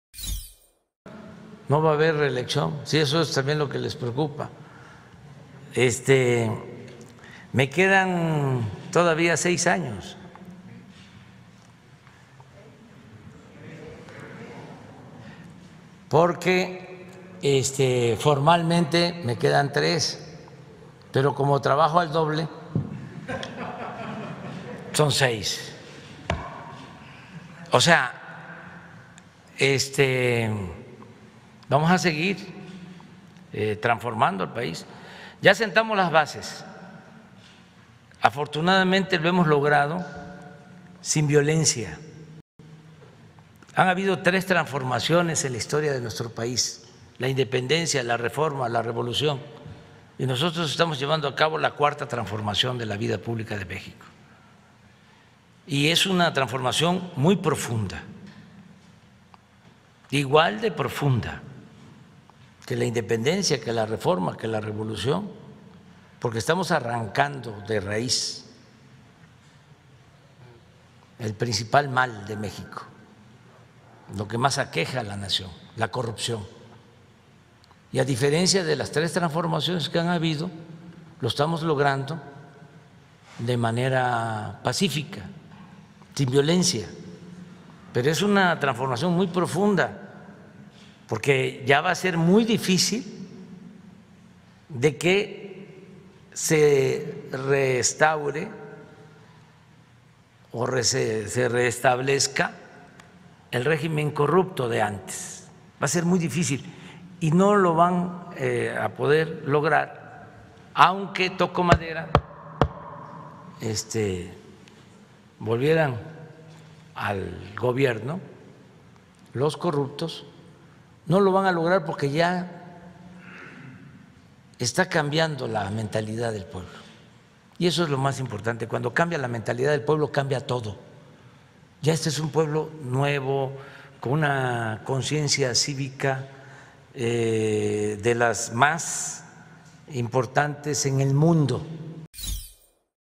Serán tres o seis años: AMLO bromeó sobre su continuidad en el Gobierno y la posible reelección
El presidente dio indicios de a quien apoyaría para las elecciones a presidencia.